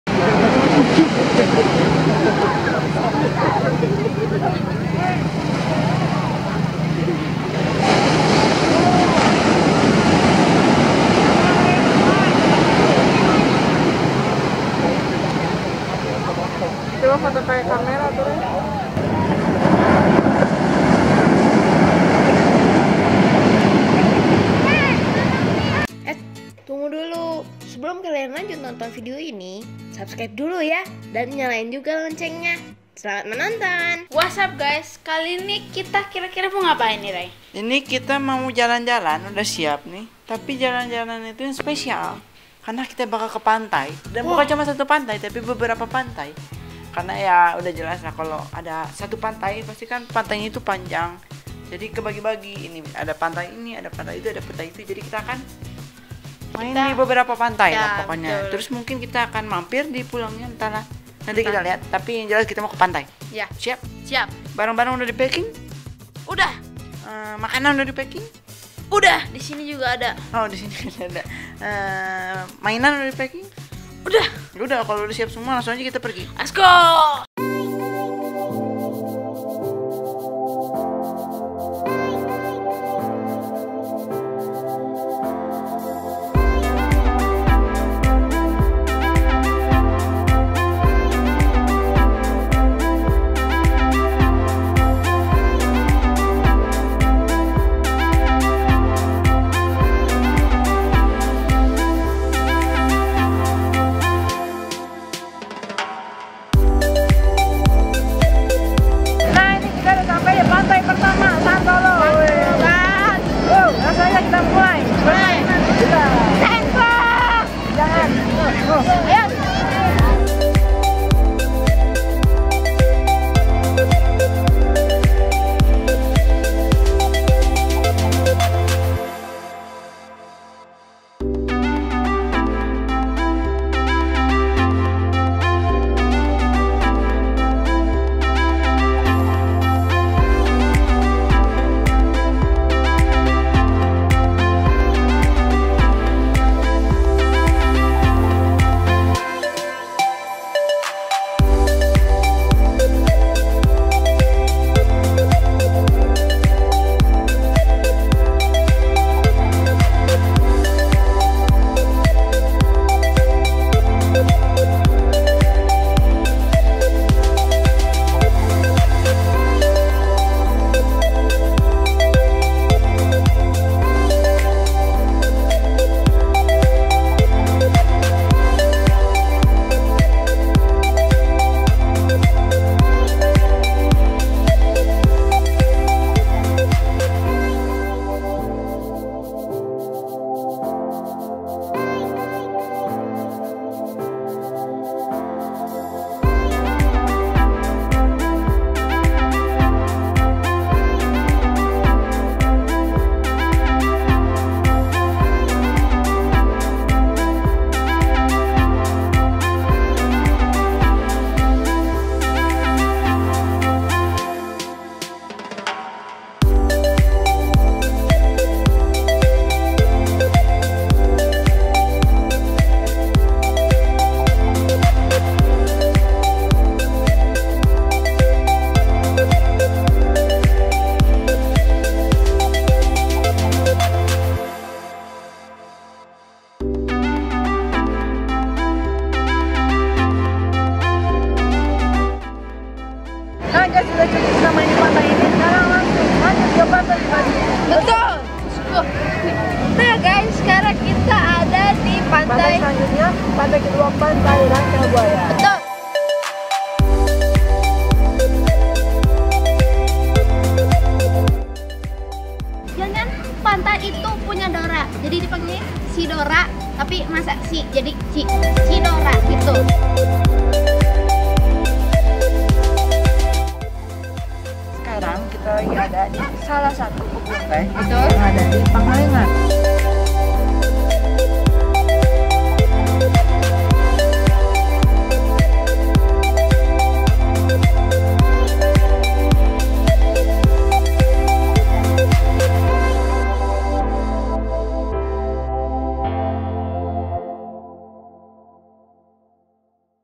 Refreshing Ke Pantai Santolo (Ternyata...) | Daily Vlog
Kali ini kita akan jalan-jalan ke Pantai Santolo. Meskipun masih masa transisi, tapi pantai yang ada di Kabupaten Garut ini udah banyak pengunjungnya lho!! Pemandangannya juga bagus, cocok nih buat kalian yang udah kangen sama jalan-jalan. Yuk langsung aja tonton videonya